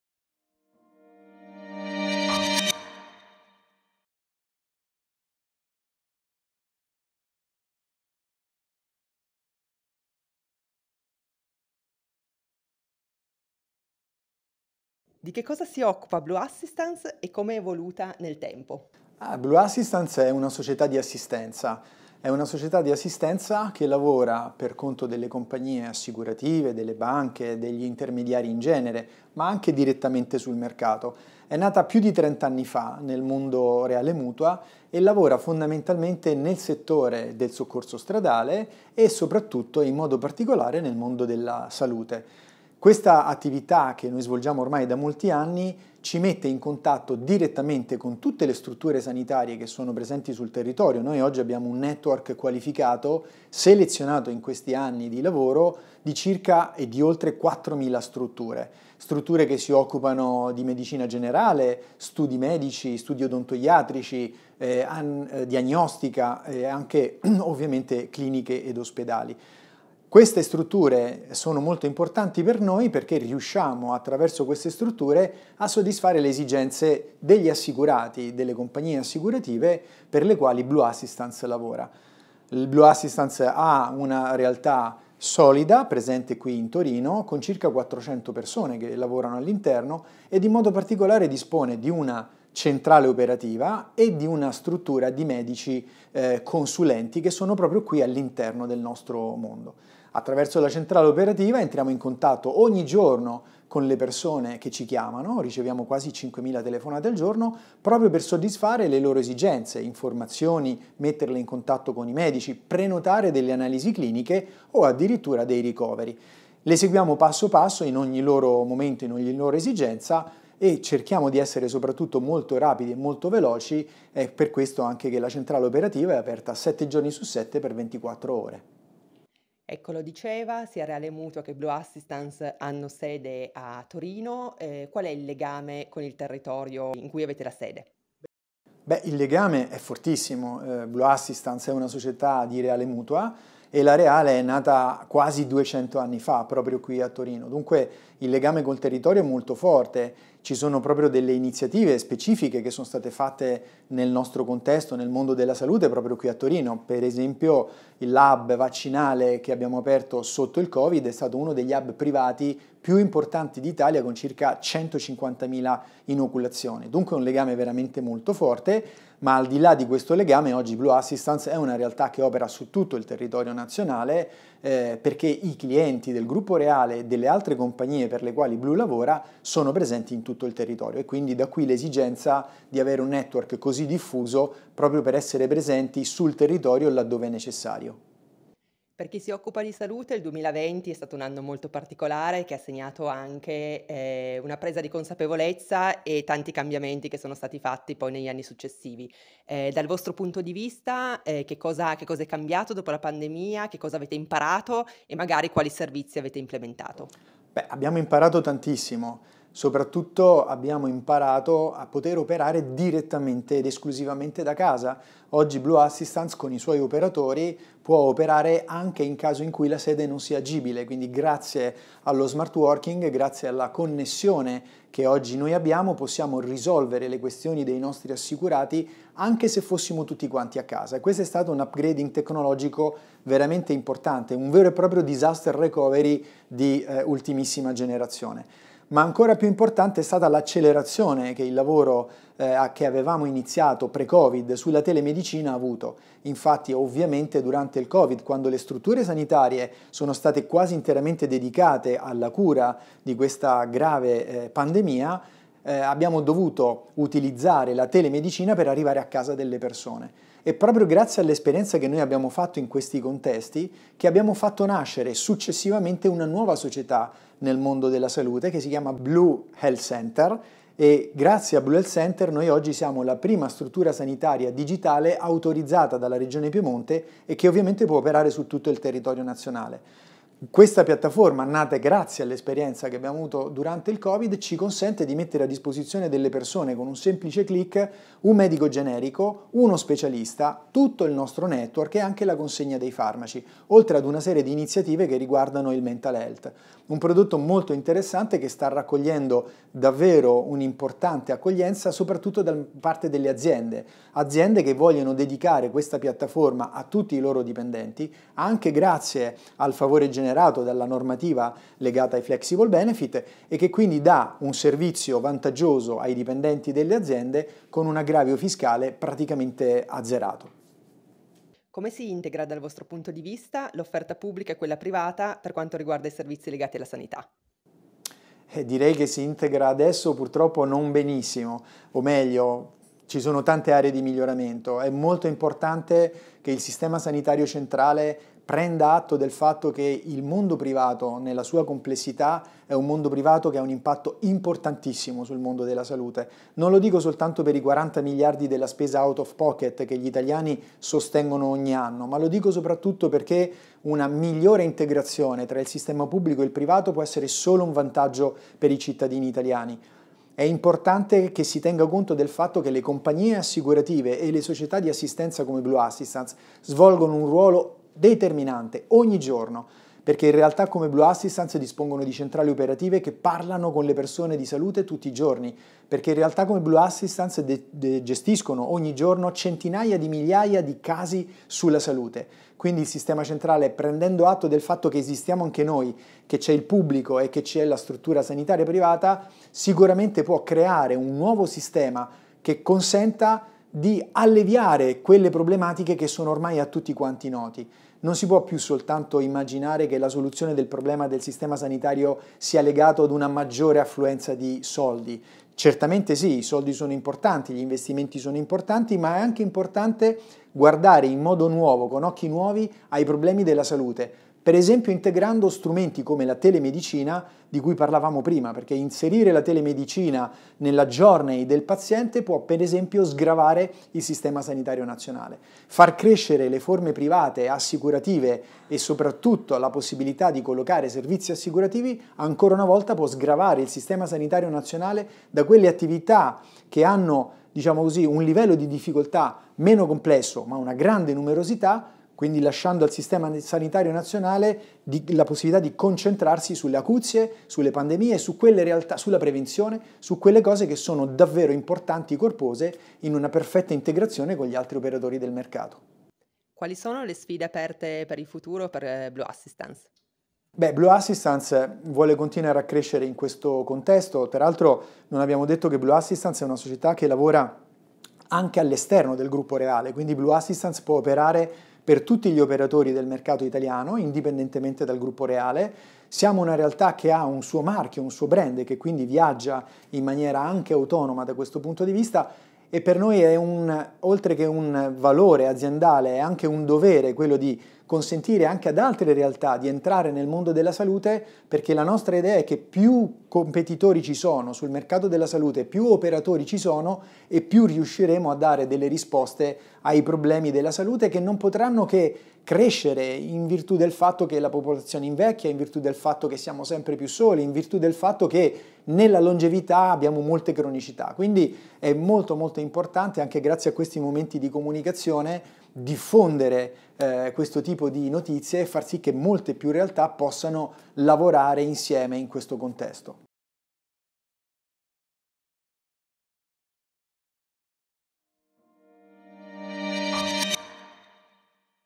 Il ruolo strategico dei service provider nel nuovo ecosistema Salute
Marco Mazzucco, Amministratore delegato Blue Assistance e Direttore Vita e Welfare Reale Mutua, racconta del network di 4.000 strutture verso le quali vengono inviate le circa 5.000 persone che ogni giorno si rivolgono a Blue Assistance per ricevere assistenza.
Mazzucco ha poi parlato delle aree di miglioramento che esistono per favorire l'integrazione tra pubblico e privato in sanità, una delle sfide fondamentali per il futuro.